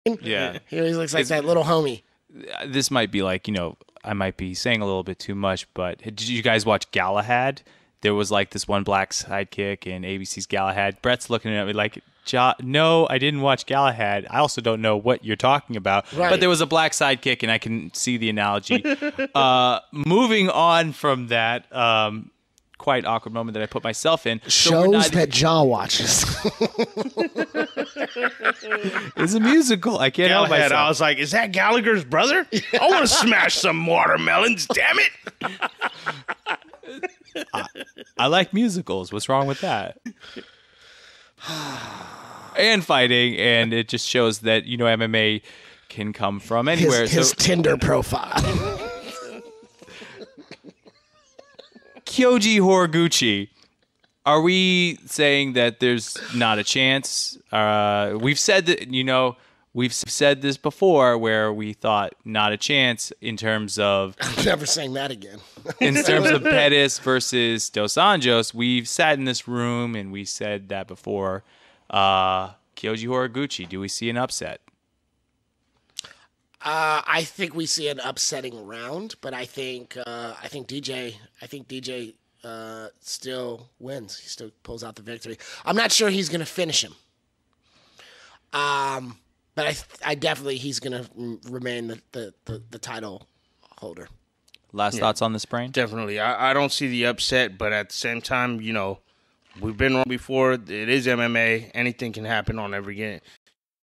UFC186TrailerVideo
Snippet from the latest UFC 186 podcast by Cage Against The Machine.